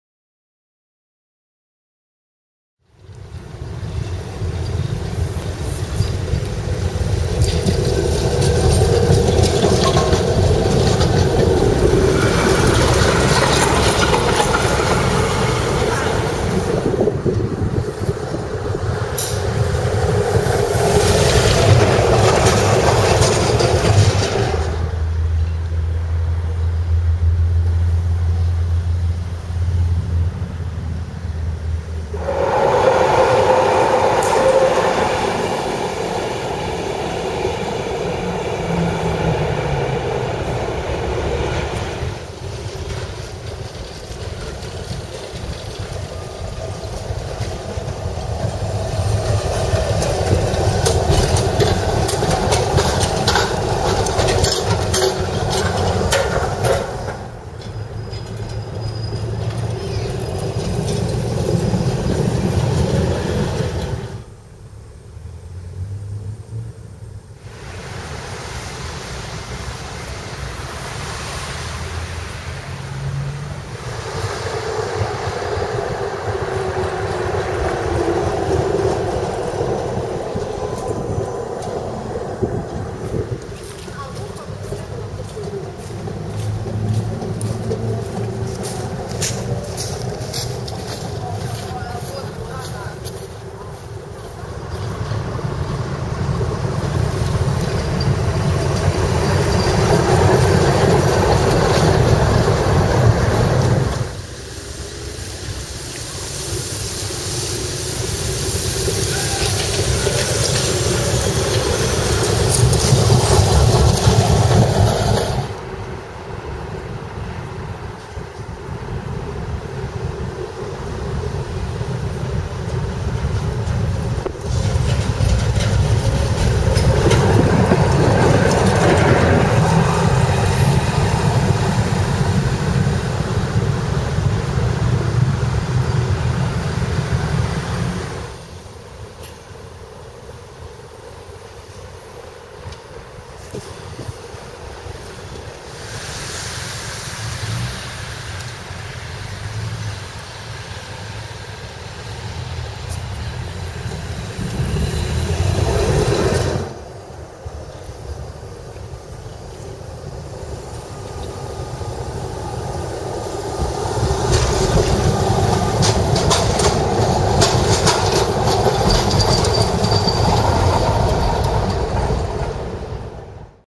Budapest's tram line 2 in the night
Nightly shots of Budapest's tram line 2, running on the left side of the Danube, right through the heart of the city. More informations about this line:

Sorry for the occasional wind noise - the wind was blowing so strong during one of the nights that it almost tripped my tripod over...